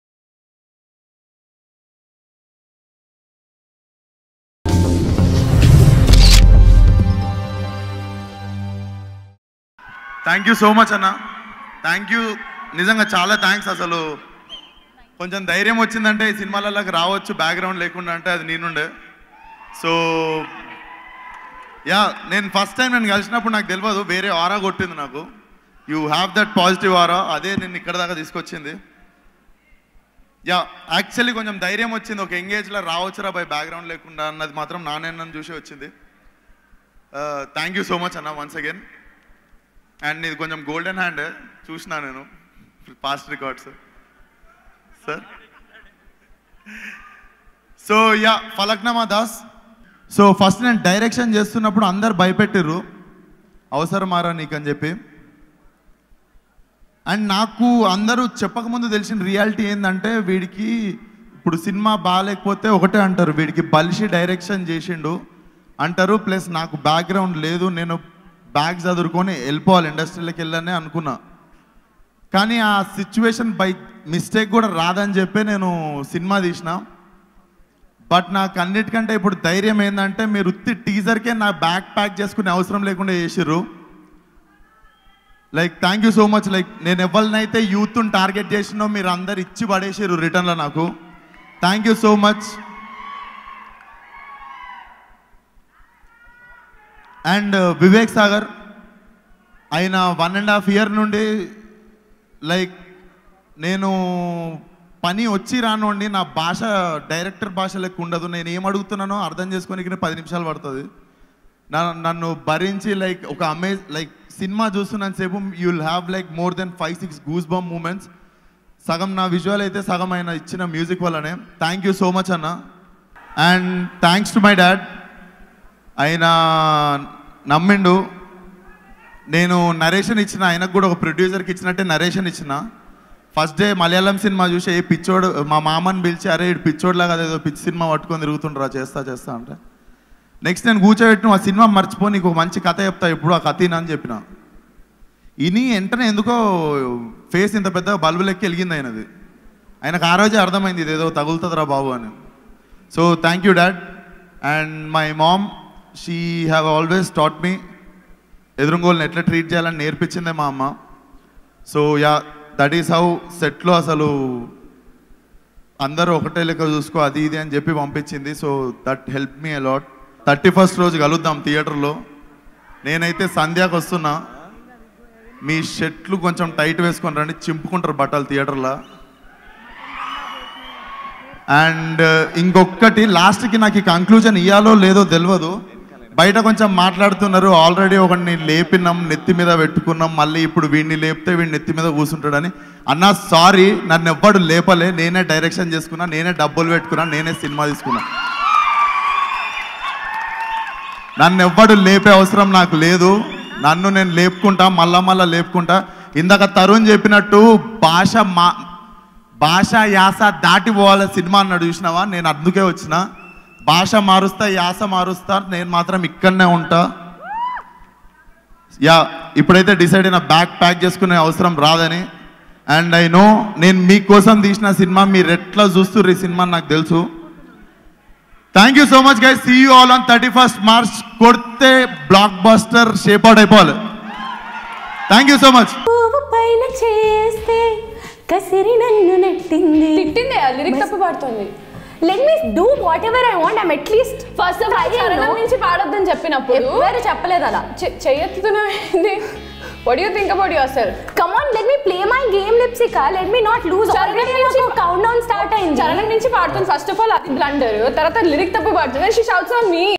Vishwak Sen Super Emotional Speech @Falaknuma Das Movie Pre Release Event - Filmyfocus.com
Vishwak Sen Super Emotional Speech @Falaknuma Das Movie Pre Release Event.